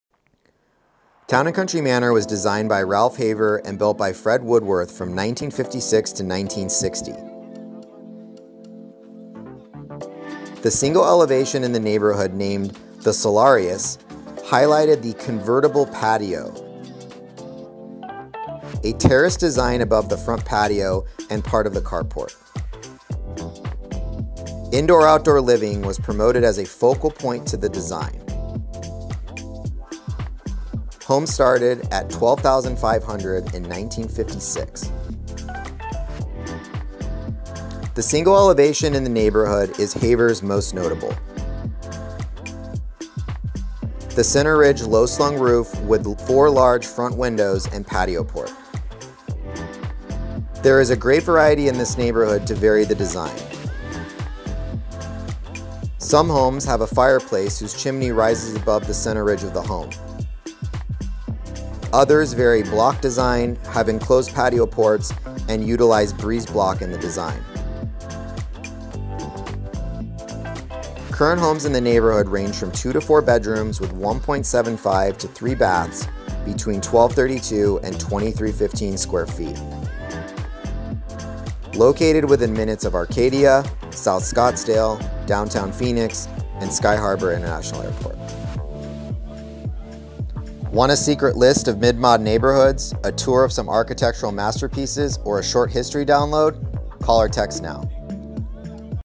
Town & Country Manor by Ralph Haver
Mid Mod Phoenix Featured Neighborhood Series
Town & Country Manor by Ralph Haver, built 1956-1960

See and experience the neighborhood and surrounding area first hand to see if this neighborhood may be the location for your next home. Tremendous examples of mid century modern architecture and design.